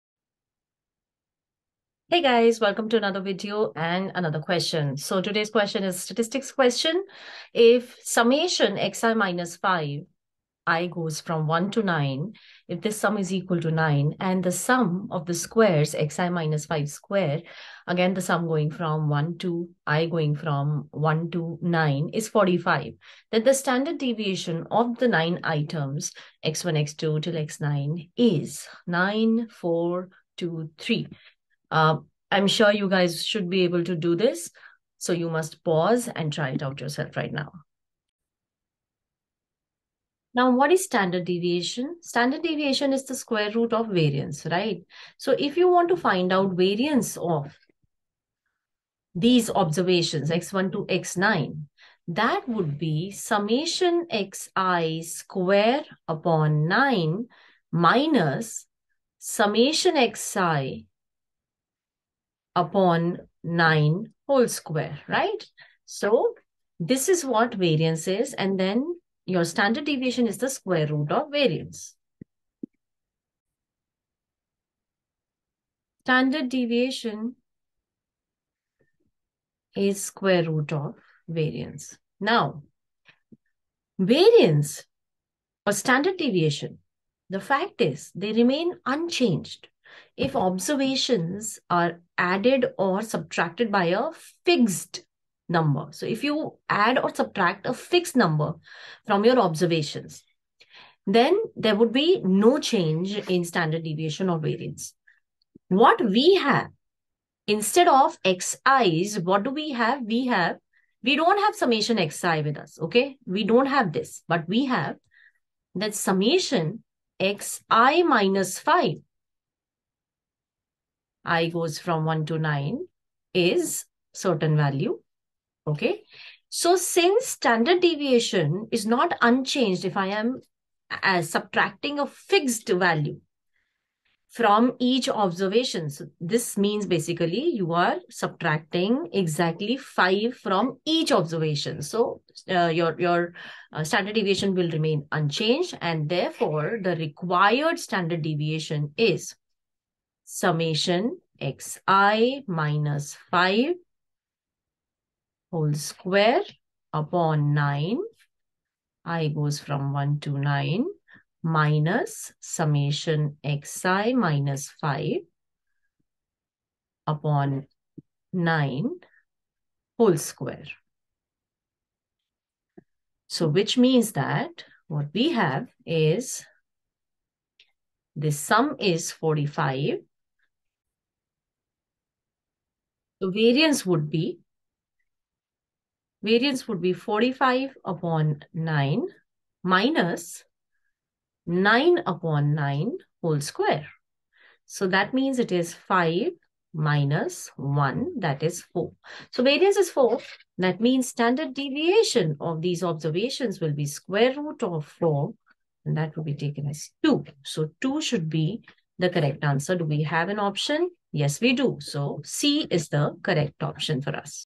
Standard Deviation when Std Dev is unchanged |CUET PG Economics 2023|CUET MA Economics Entrance|Q55
In this MCQ, we are trying to solve MCQ of Probability with the help of numerical example. This is a practice MCQ for CUET PG Economics.  This is also useful for those who are preparing for Indian economic services and UGC NET Economics. Please solve it and let us know your answer in a comment.

1) Basics of MA Economics Entrance Exam (DSE/ISI/JNU/IGIDR/MSE)
2) Semester Exam, Economics H
3) UGC Net Economics
4) Indian Economic Services